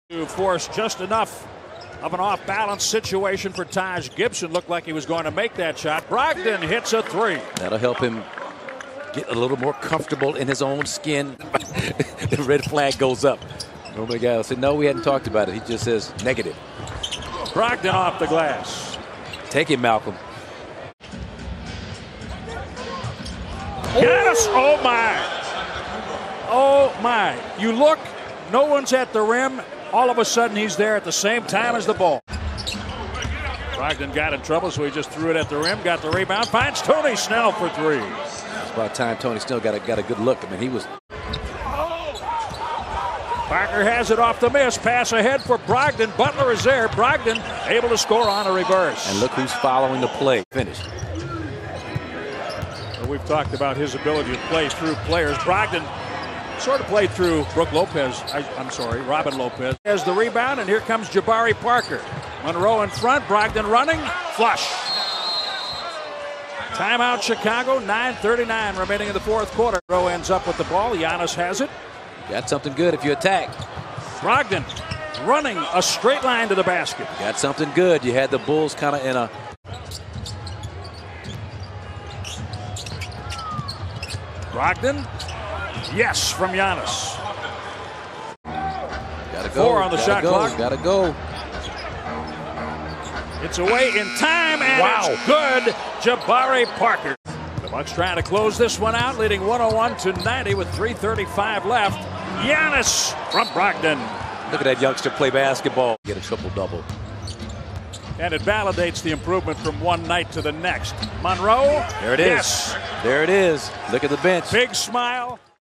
Malcolm Brogdon Records First Triple Double In The NBA
Here is Malcolm Brogdon recording his first ever triple double in the NBA.